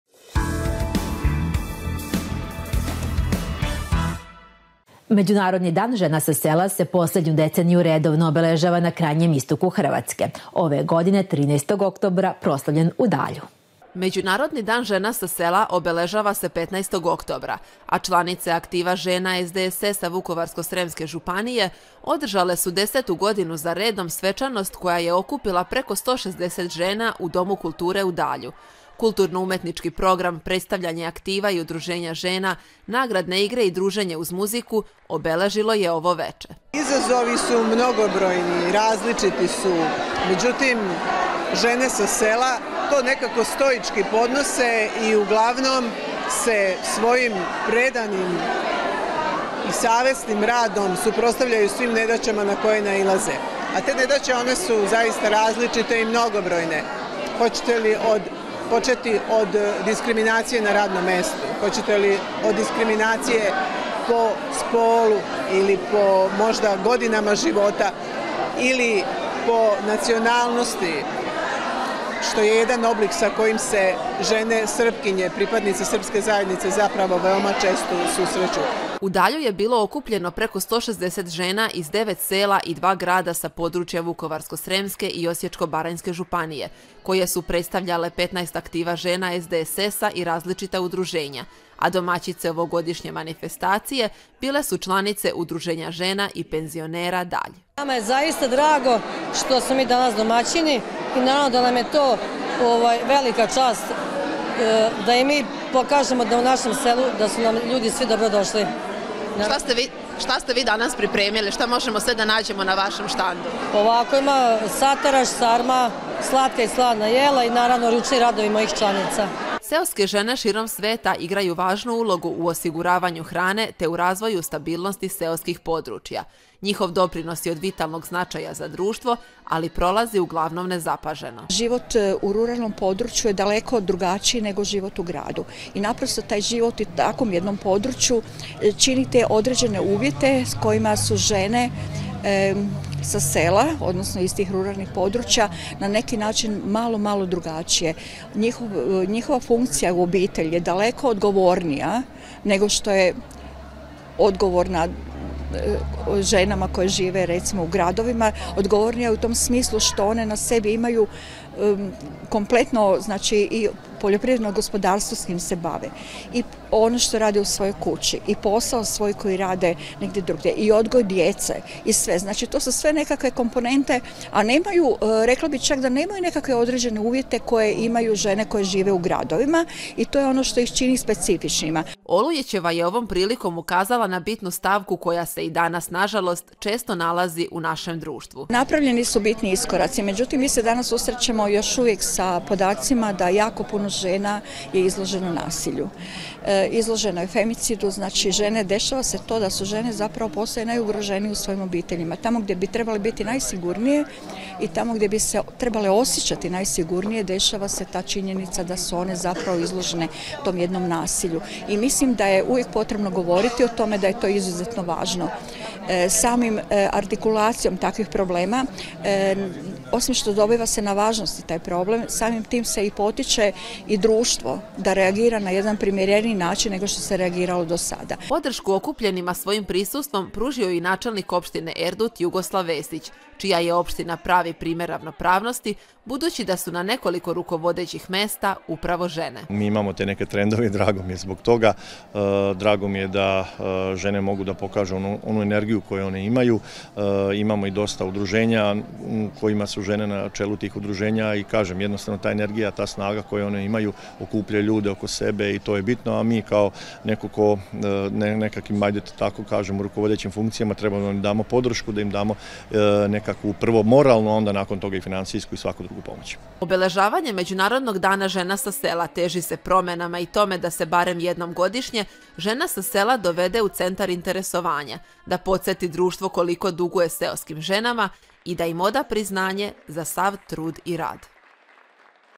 HRONIKA SBIZS 451: Jubilej obeležavanja Međunarodnog dana žena sa sela
Aktiv žena Samostalne demokratske srpske stranke Vukovarsko-sremske županije sinoć je u Dalju proslavio Međunarodni dan žena sa sela.

Međunarodni dan žena sa sela obeležava se 15. oktobra, a članice Aktiva žena SDSS-a Vukovarsko-sremske županije održale su, desetu godinu za redom, svečanost koja je okupila preko 160 žena u Domu kulture u Dalju. Kulturno-umetnički program, predstavljanje aktiva i udruženja žena, nagradne igre i druženje uz muziku obeležilo je ovo veče.

- Ovo je prilika da se bar na trenutak skrene pažnja na sve one poslove, izazove i probleme sa kojima se susreće jedna seoska žena, bilo prijašnjih godina ili u ovo savremeno doba. Izazovi su mnogobrojni i različiti, međutim, žene sa sela to stoički podnose i uglavnom se svojim predanim i savesnim radom suprotstavljaju svim nedaćama na koje nailaze. Te nedaće su zaista različite i mnogobrojne, hoćete li početi od diskriminacije na radnom mestu, diskriminacije po spolu, možda po godinama života ili nacionalnosti sa kojim se žene pripadnice srpske zajednice veoma često susreću. Tako da je 15. oktobar zaista dan koji je gotovo jedak Osmom martu, to je trenutak kada ćemo svu pažnju usmeriti na sve one vredne domaćice, radnice koje svakodnevno rade i suprotstavljaju se svim nedaćama – izjavila je saborska zastupnica Dragana Jeckov koja je ovom prilikom ukazala na položaj žena danas.

- Za mene je poražavajuća činjenica da su tek jedna trećina žena vlasnice OPG-ova, jednako tako, postoji neko nepisano pravilo da zajedničku imovinu nasleđuju muška deca, sve su to stvari koje se, uprkos razvoju i napretku društva uopšte, još uvek događaju i sa njima se još uvek susrećemo. Ono što je dobro jeste emancipacija koju žene vrednuju i bore se za nju. Danas su one rame uz rame sa gotovo svim zanimanjima sa kojima se bave i muškarci, imamo vrsne doktorke, inžinjerke, političarke. Usuđujem se to pokazati i na svom primeru kada ne dozvoljavam da me se degradira samim tim što sam žena ili, ne daj Bože, što sam Srpkinja – pojašnjava Jeckov.

U Dalju je bilo okupljeno preko 160 žena iz devet sela i dva grada sa područja Vukovarsko-sremske i Osječko-baranjske županije koje su predstavljale 15 Aktiva žena SDSS-a i različita udruženja, a domaćice ovogodišnje manifestacije bile su članice Udruženja žena i penzionera Dalj.

- Nama je zaista drago što nam je pripalo domaćinstvo ove manifestacije, velika nam je čast i prilika da pokažemo kako su svi dobrodošli u naše selo. Na našem štandu mogu se videti i probati sataraš, sarma, slatka i slana jela i naravno, izloženi su ručni radovi naših članica – kaže predsednica Udruženja žena i penzionera Dalj Branka Sundić.

Seoske žene širom sveta igraju važnu ulogu u osiguravanju hrane, te u razvoju stabilnosti seoskih područja. Njihov doprinos je od vitalnog značaja za društvo, ali prolazi uglavnom nezapaženo.